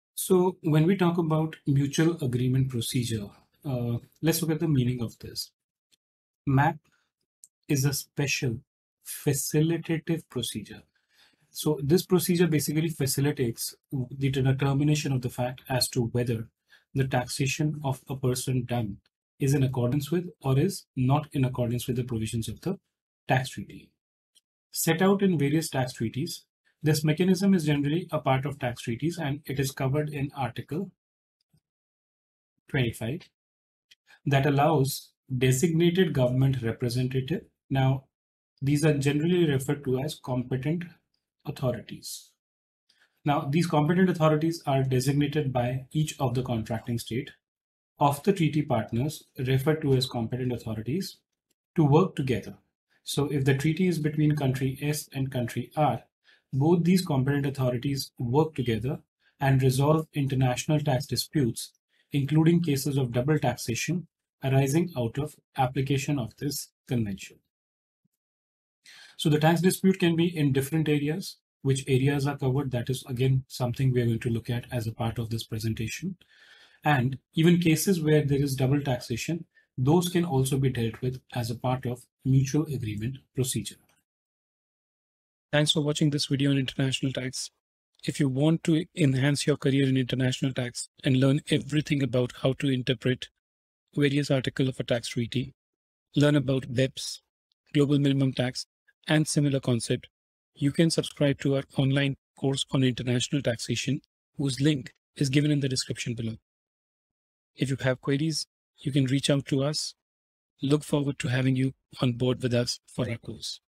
Mutual Agreement Procedure (MAP) Meaning - Article 25 of DTAA - International Tax +91-9667714335
As per Article 25, Mutual Agreement Procedure (MAP) is a special facilitative procedure set out in various tax treaties that allows Competent Authorities of Treaty partners to work together, and resolve  international tax disputes.

Mutual Agreement Procedure Meaning - International Taxation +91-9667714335 - CA Arinjay Jain